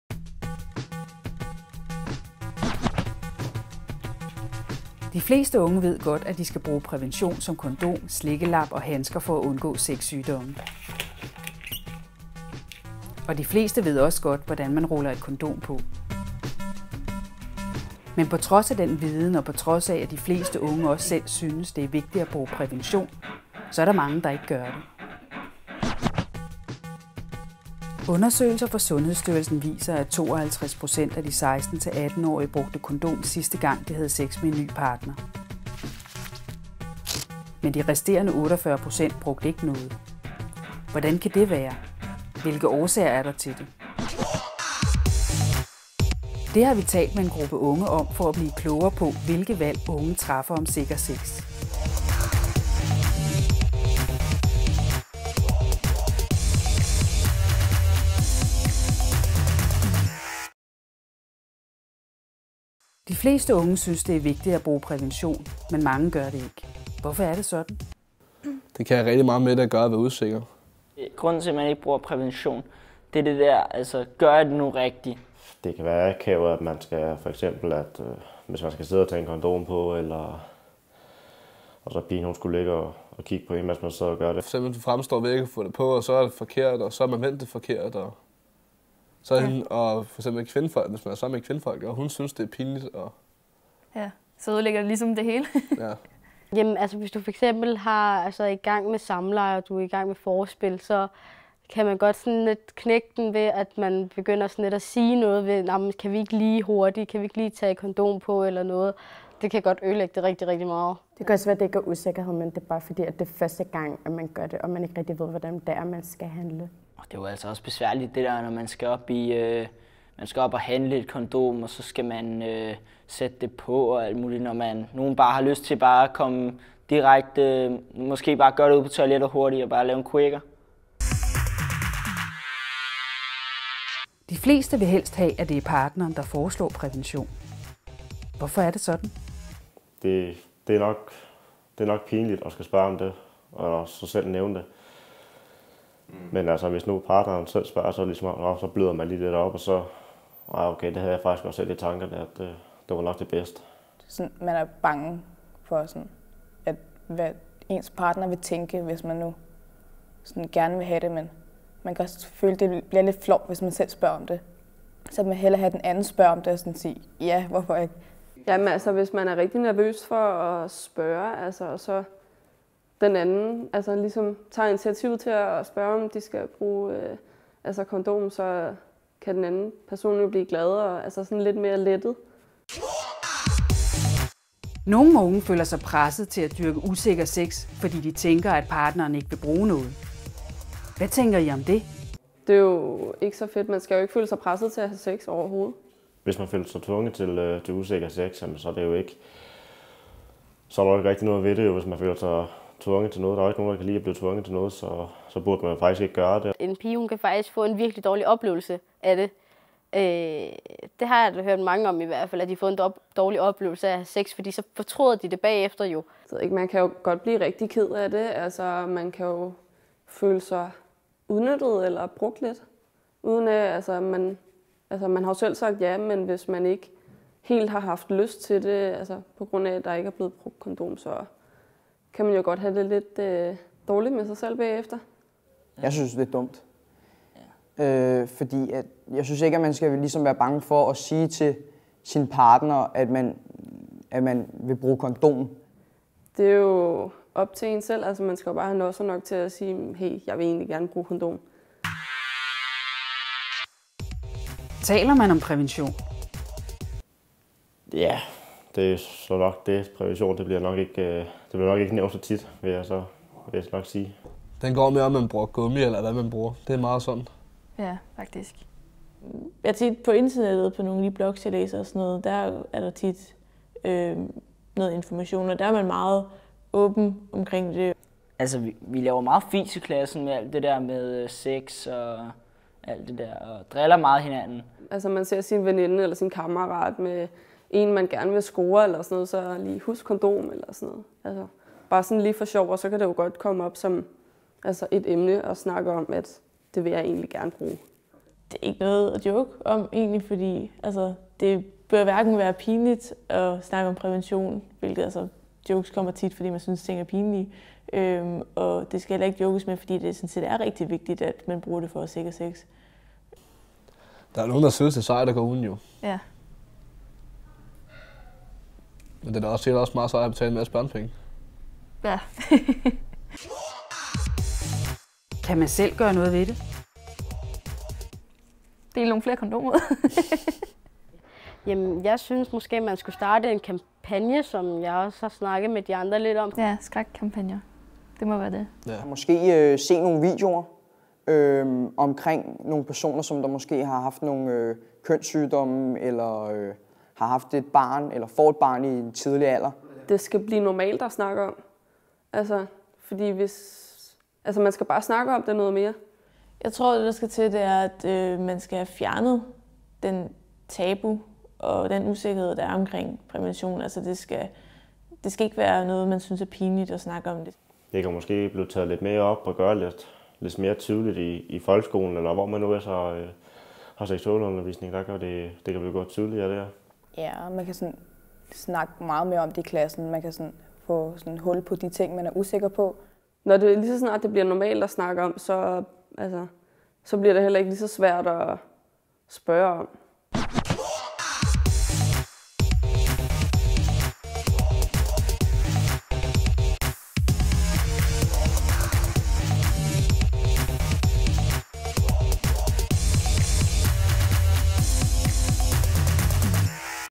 Sikker sex - hvorfor ik´?
De fleste unge synes, det er en god ide at dyrke sikker sex, men rigtig mange gør det alligevel ikke. Hvorfor er det sådan? Hør hvad de unge selv mener.